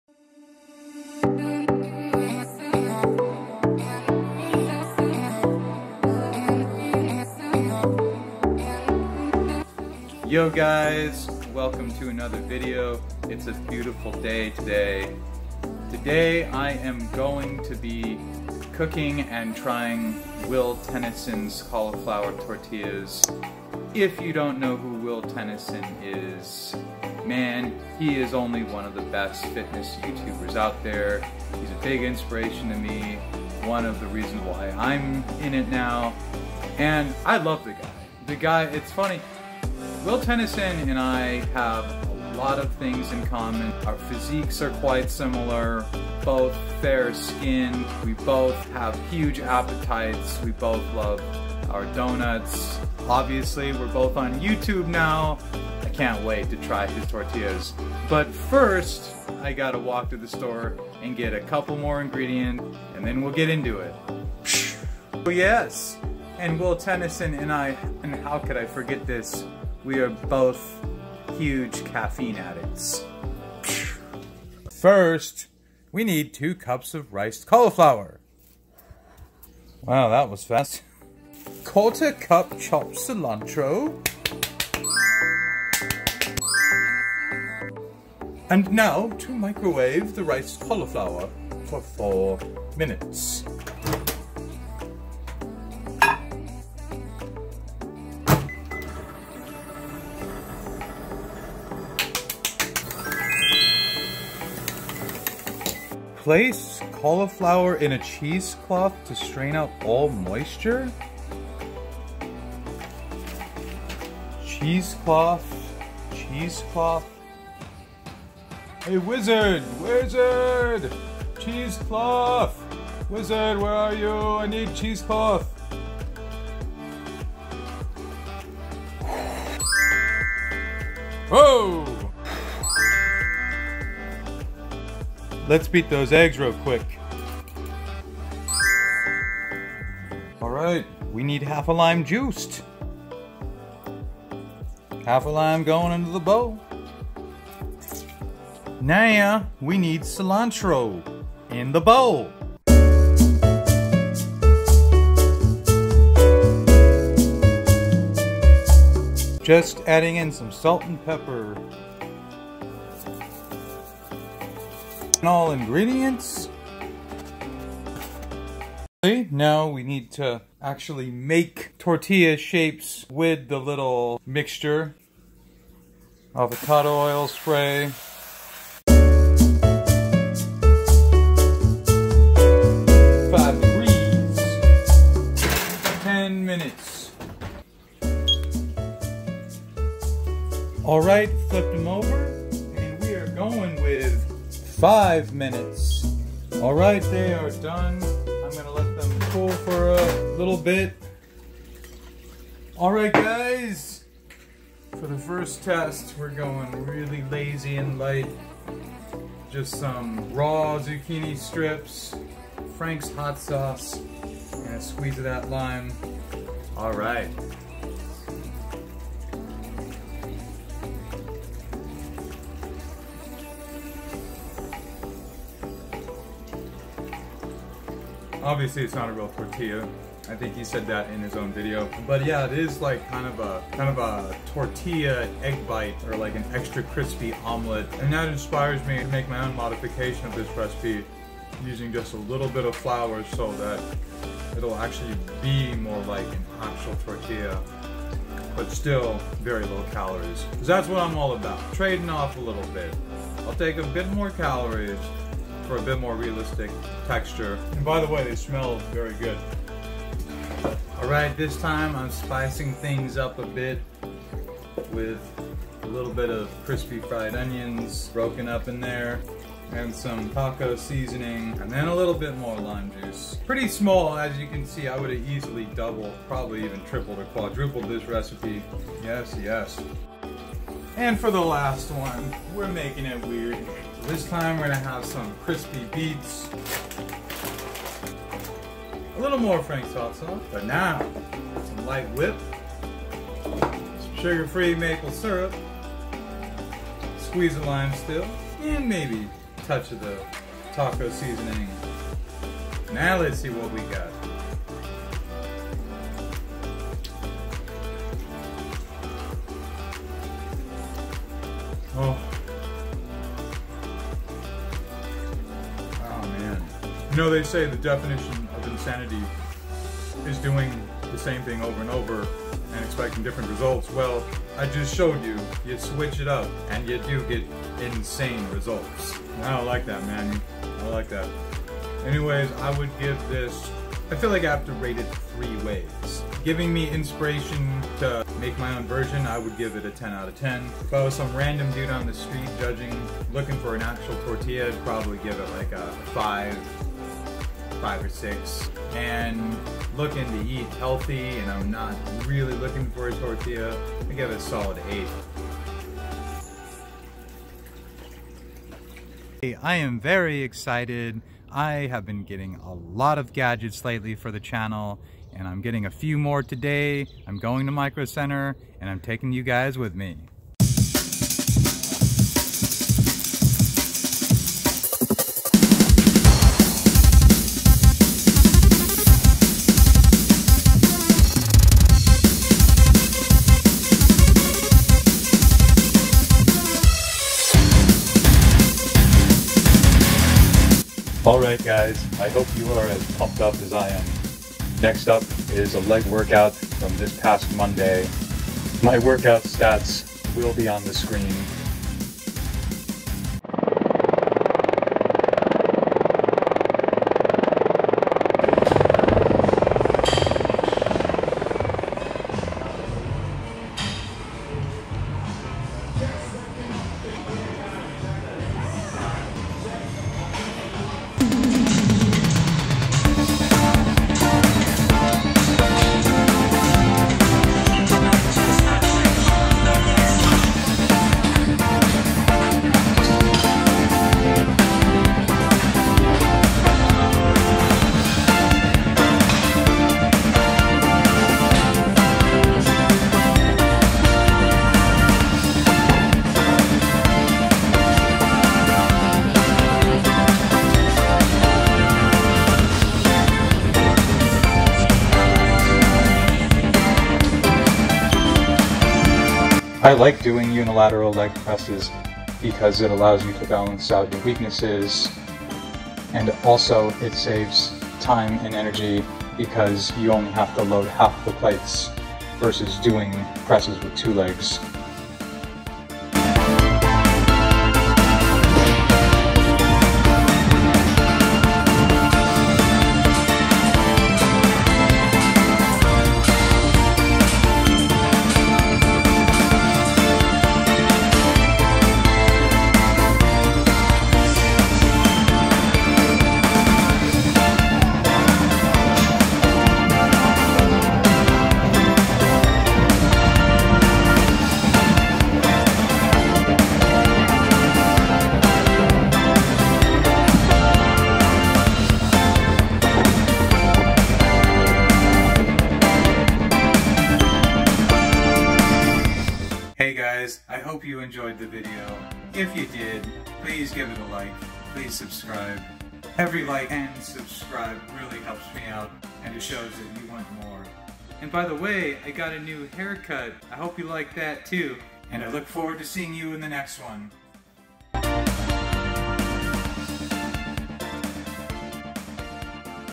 Trying Will Tennyson's Cauliflower Tortillas. Leg Day at the Gym. Timelapse. Driving.
Aerial footage from my new drone.

Heavy Metal Music: Manuel Senfft (Tagirijus)